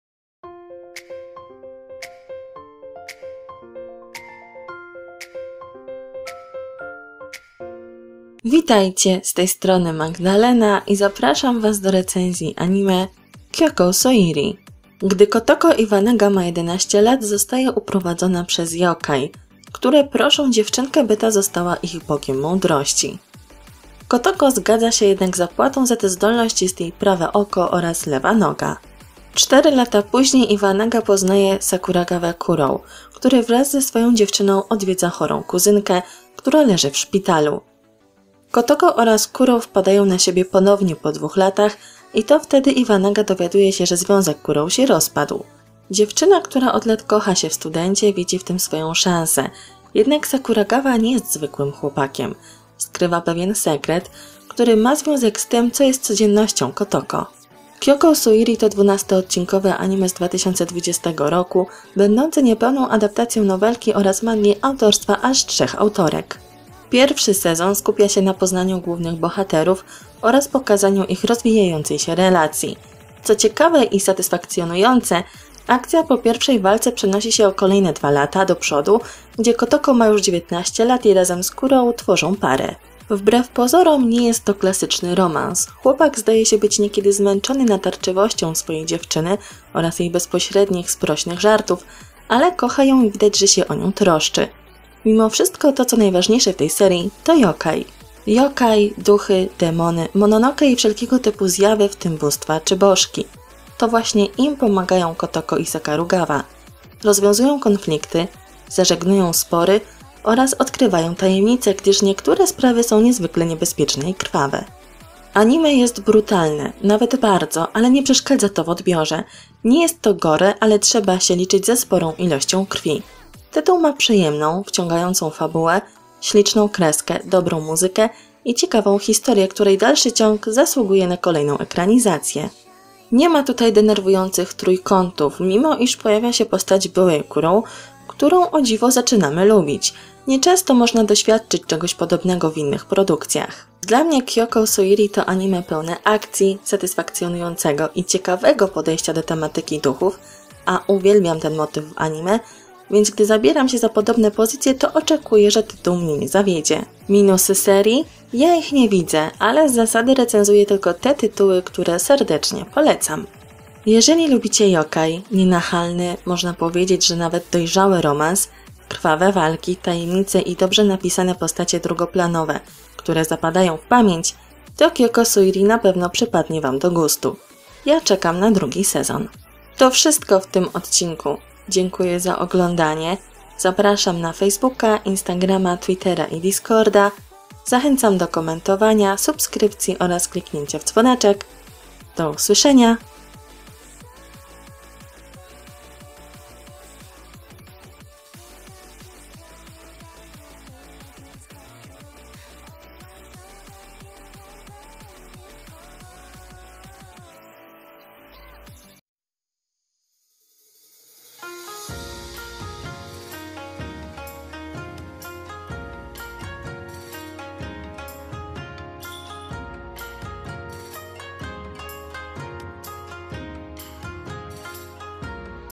ZWIĄZEK Z PRZYPADKU I DEMONY! Recenzja Anime "Kyokou Suiri" 🌸😈
Witajcie! 🌸 Mix romansu i demonów! :)